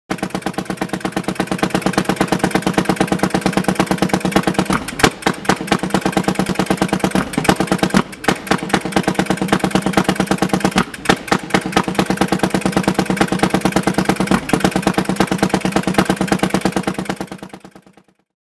Solo Engine
This Solo Engine from the Skoog brothers engine factory in Borlänge, Sweden, was built in the 1930s. It is thought to have been mobile and used to drive early farmwork machinery such as threshers, log splitters and circular saws. At startup the engine runs on gasoline and ignites via a spark plug, but when heated it runs mainly on kerosene, which was a cheaper type of fuel in the time when the engine was used. The Solo Engine is a two stroke engine and has a total engine power capacity of 7 horsepower.
Recorded at Jäders bruk